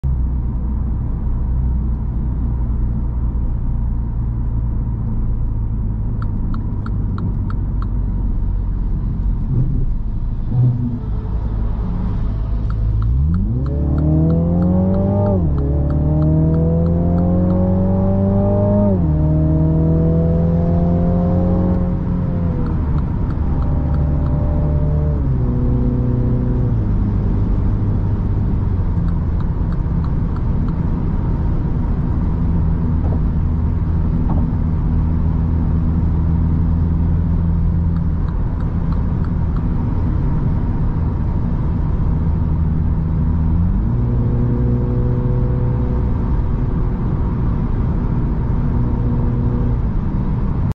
😎 Mini Cooper Clubman F54 - 306hp Test drive
Mini Cooper Clubman

The Mini Cooper Clubman is an estate car developed by the British manufacturer Mini. It is the first vehicle of this type in the Mini range to see the light of day since the takeover of the brand by the BMW Group. The name first appeared on a 3-door GT version of the Mini in 1959.

What is your favorite car?
What is your dream car?

Take care of yourself and your car! 🙏